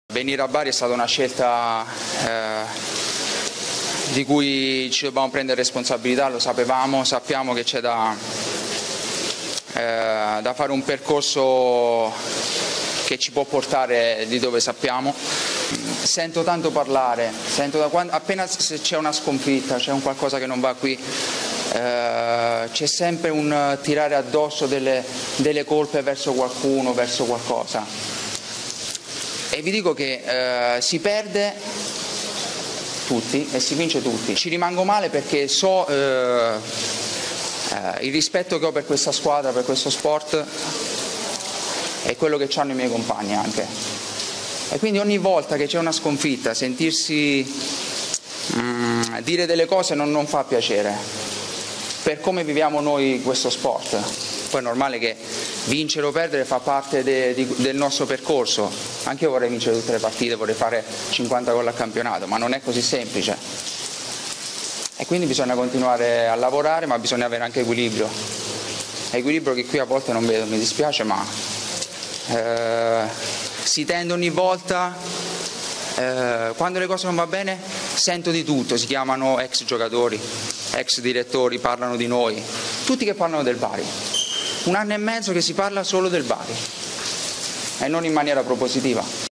Bari calcio, Antenucci: "Quando le cose vanno male, qui succede di tutto: ci vuole più equilibrio"
Lo sfogo del bomber biancorosso, autore del gol decisivo contro il Catanzaro